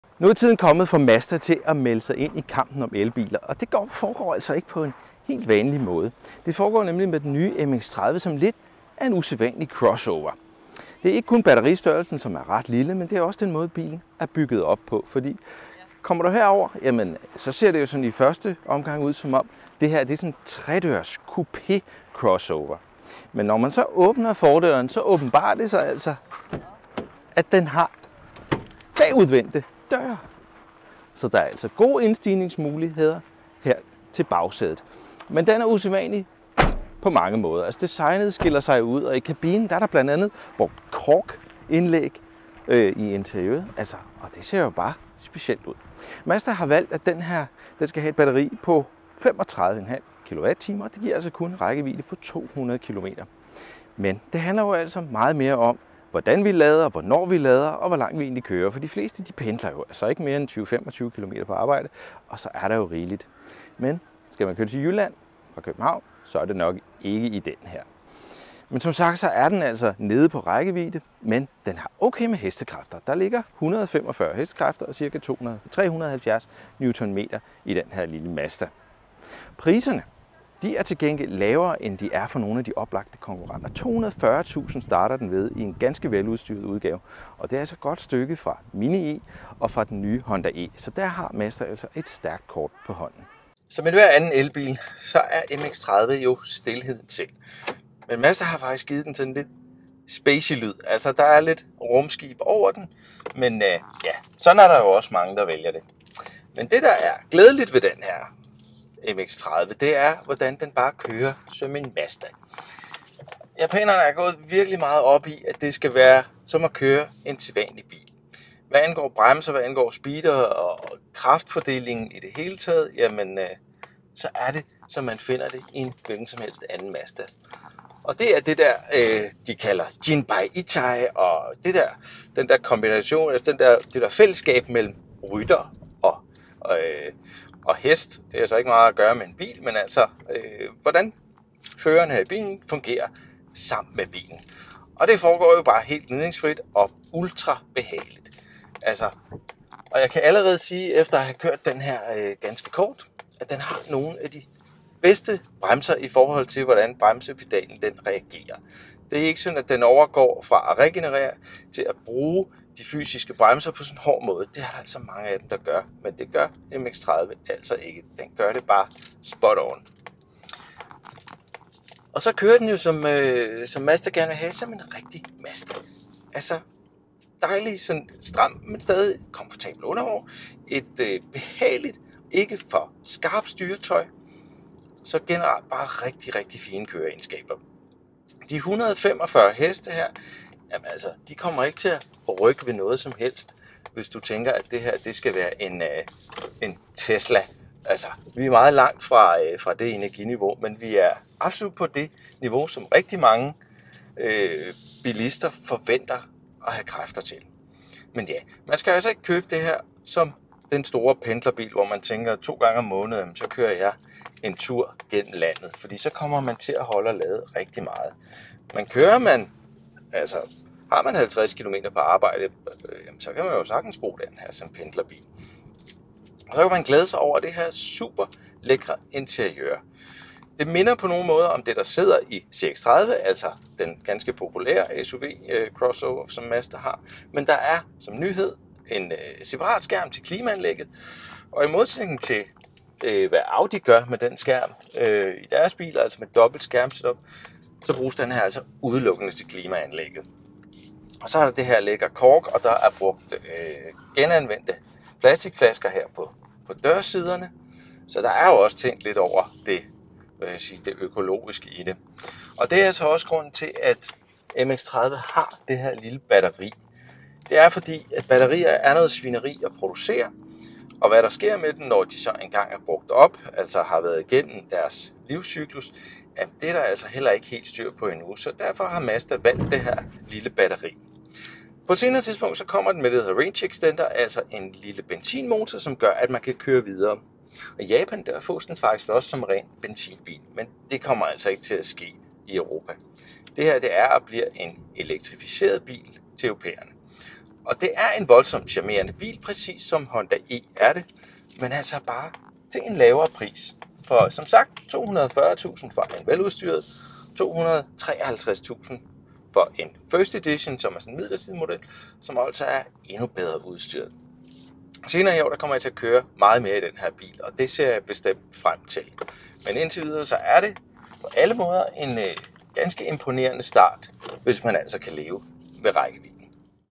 FIRST DRIVE - Mazda MX-30 har charmen (ikke så meget rækkevidden)
Den er billigere end Mazda 3, har skjulte bagdøre som vender bagud og der er brugt kork som en del af interiøret: Mazda MX-30 er japanernes første bud på en helt elektrisk bil, og den er både charmerende, smart og helt sin egen.

Men så er der rækkevidden. 200 kilometer er, hvad den kan blive til, men Mazda har sine grunde - hør om dem og meget andet, når vi sætter os bag rattet af den elektriske japaner.